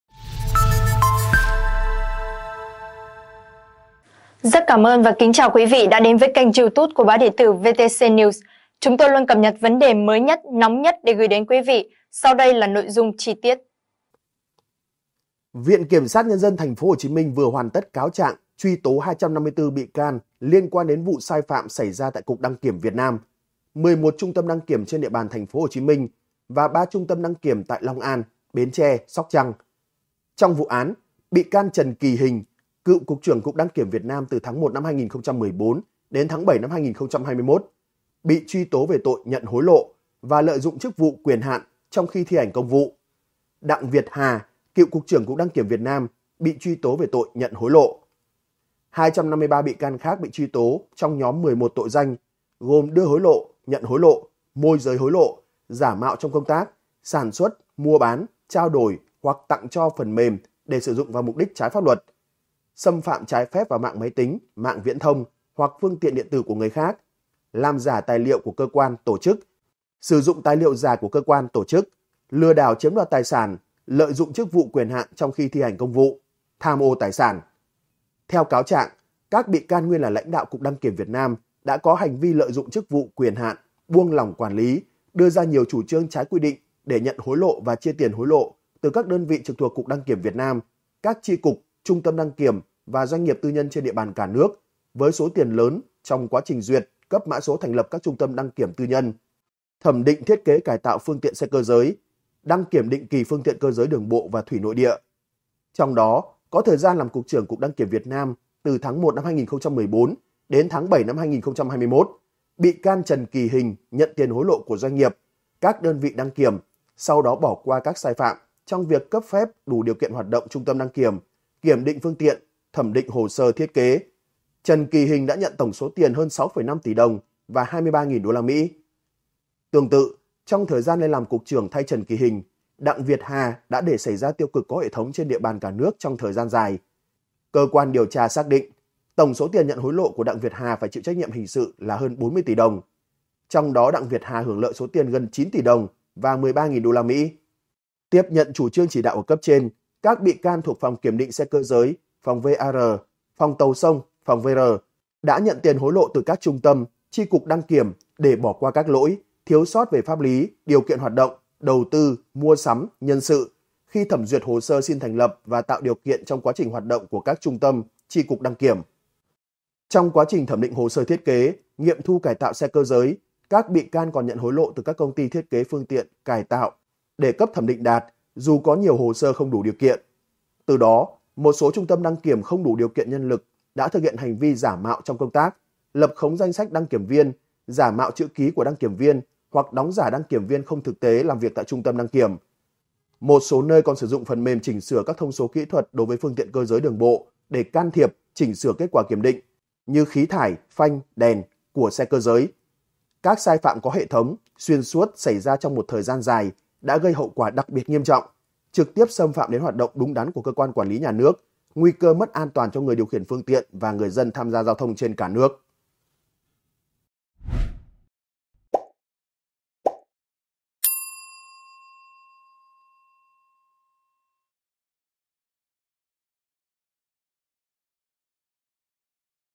‘Đút túi’ hơn 40 tỉ đồng tiền ‘bôi trơn’, 2 cựu Cục trưởng Cục Đăng kiểm bị truy tố
Tin tức mới nhất | ‘Đút túi’ hơn 40 tỉ đồng tiền ‘bôi trơn’, 2 cựu Cục trưởng Cục Đăng kiểm bị truy tố
VKSND TP.HCM vừa hoàn tất cáo trạng, truy tố 254 bị can liên quan đến vụ án sai phạm xảy ra tại Cục Đăng kiểm Việt Nam, 11 trung tâm đăng kiểm trên địa bàn TP.HCM, và 3 trung tâm đăng kiểm tại Long An, Bến Tre, Sóc Trăng.Trong vụ án, bị can Trần Kỳ Hình (cựu Cục trưởng Cục Đăng kiểm Việt Nam từ tháng 1/2014 - 7/2021) bị truy tố về tội Nhận hối lộ và Lợi dụng chức vụ, quyền hạn trong khi thi hành công vụ; Đặng Việt Hà (cựu Cục trưởng Cục Đăng kiểm Việt Nam) bị truy tố về tội Nhận hối lộ.